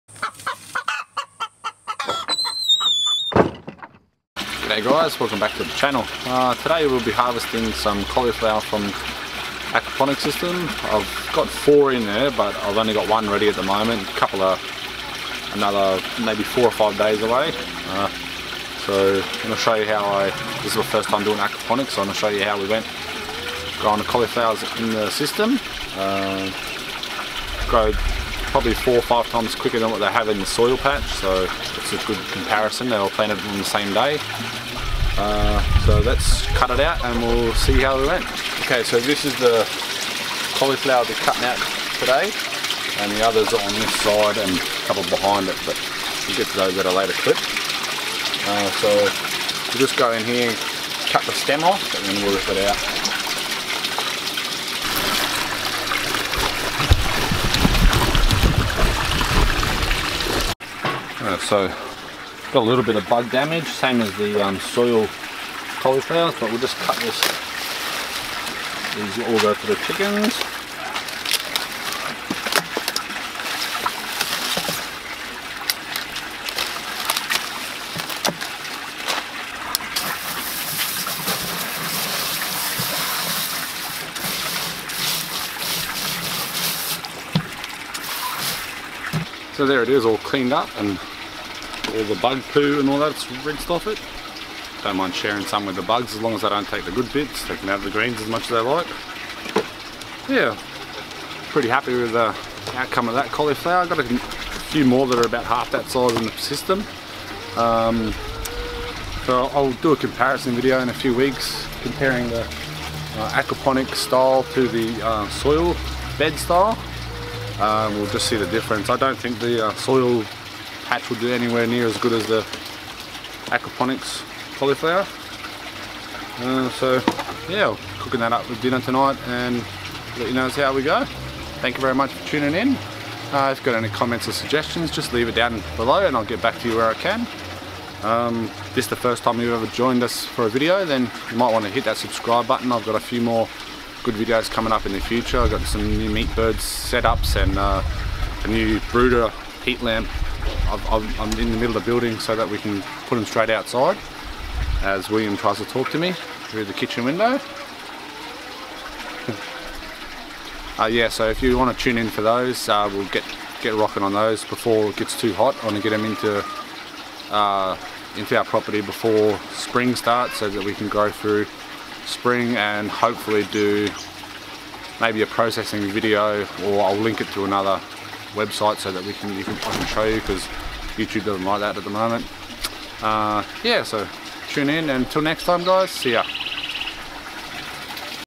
first aquaponic cauliflower harvest
hey everyone,

in this short video i show myself harvesting the very first cauliflower ready to be picked from the aquaponics set up.

i can do a comparison in a few weeks comparing soil garden bed grown cauliflowers vs the aquaponic grown cauliflowers.